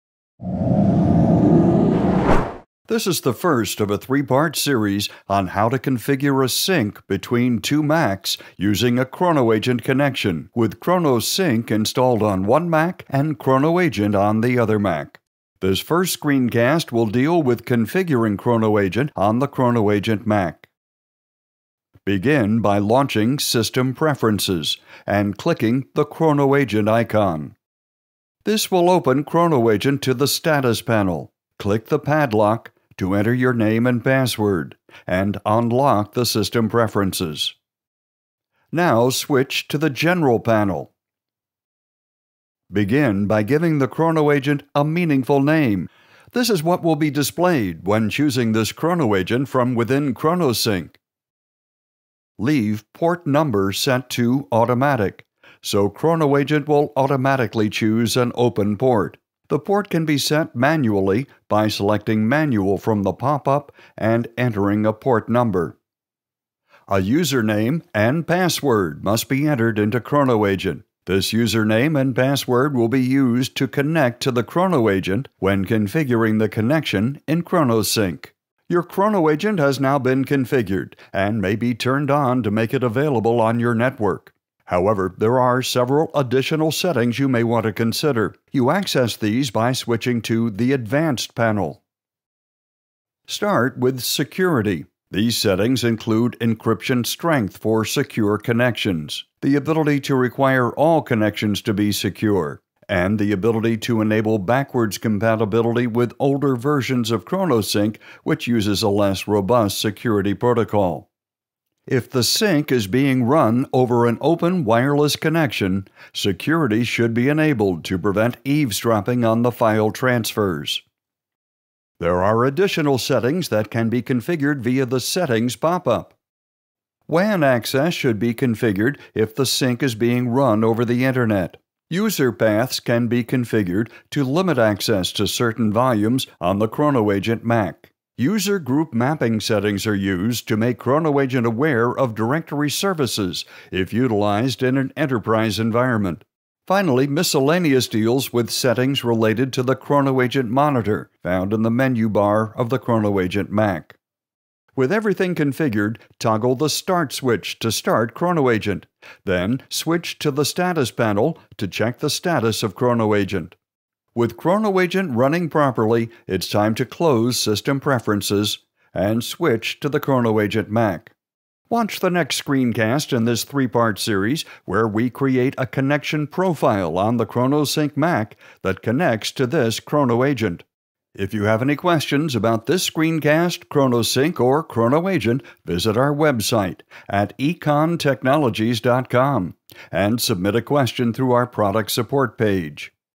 Configuring a ChronoAgent Sync Part I
This screencast shows you how to configure ChronoAgent on your ChronoAgent Mac